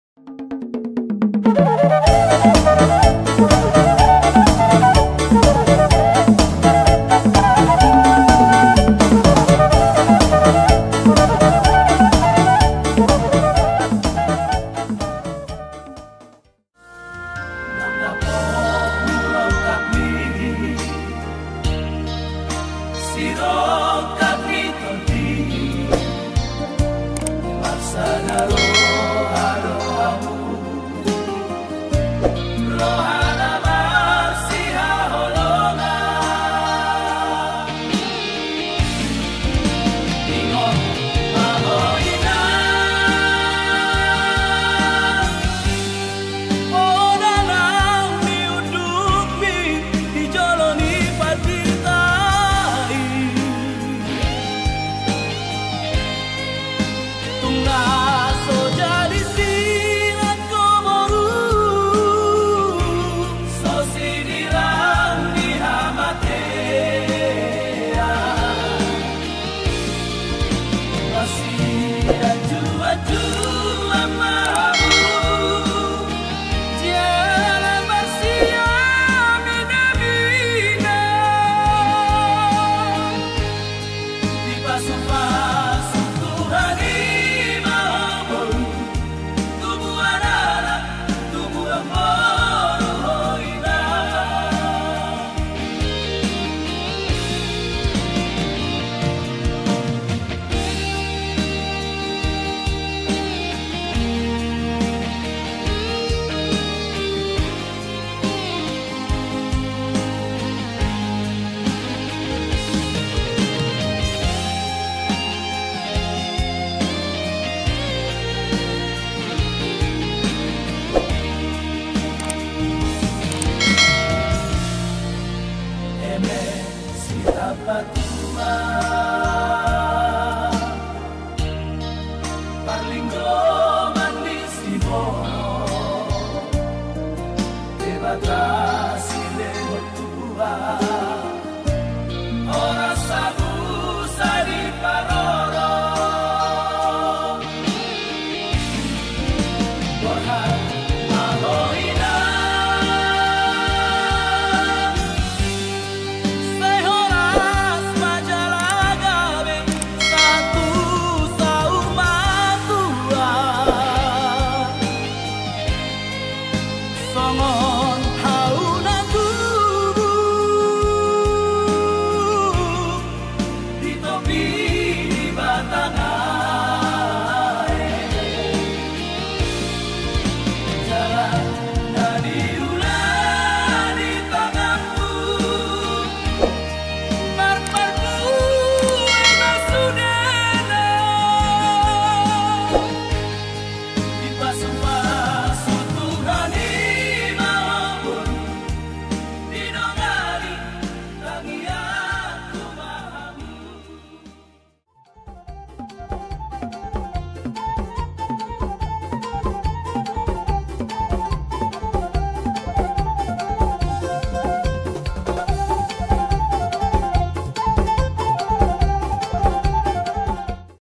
Prosesi Ulos Hela - Pernikahan Adat Batak Agatangelus & Anggita
Happy Wedding
Pernikahan 20 Januari 2024
Tempat Gd. Gorga 2

- Instagram : @bjesmusik & @bjesdancer.ofc
- Facebook : Alunan Khas Pesta